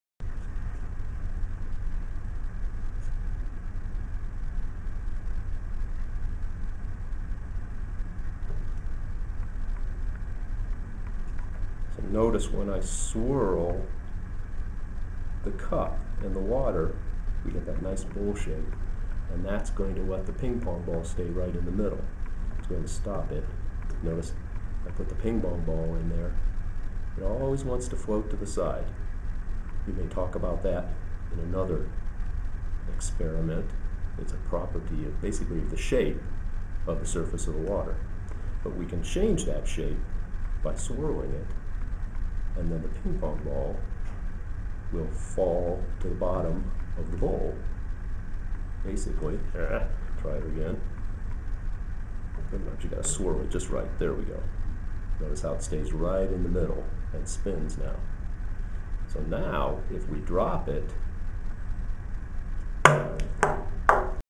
Ping Pong Ball Blaster (close-up)
Closer view of the concave shape of the water surface after swirling the cup.  The full experiment is demonstrated in this video , and described in more detail on our
BTW- bet your friends to try and make a ping pong ball stay in the center of a cup of water (or whatever your drink may be), before showing them the trick of course!